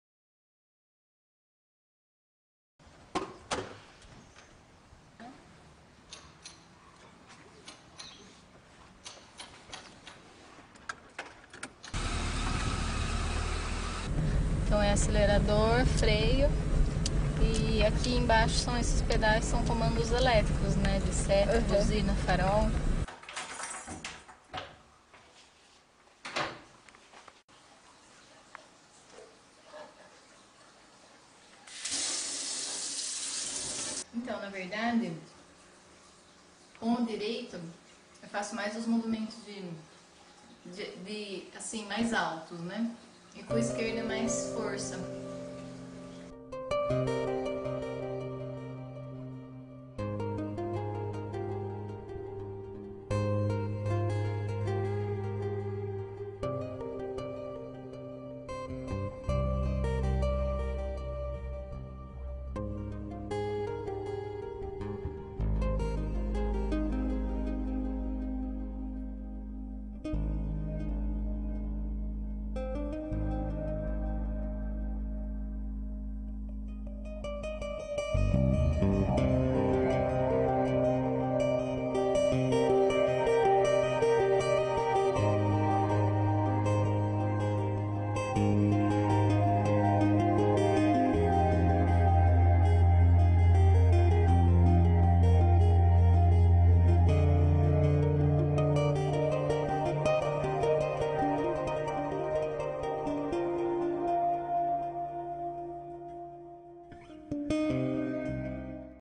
Woman with no arms - NO EXCUSES
Woman with no arms who does everyday normal things others would would using their arms or hands like eating, brushing hair, putting on  make up, using a computer, making phone calls, opening doors and even driving a car.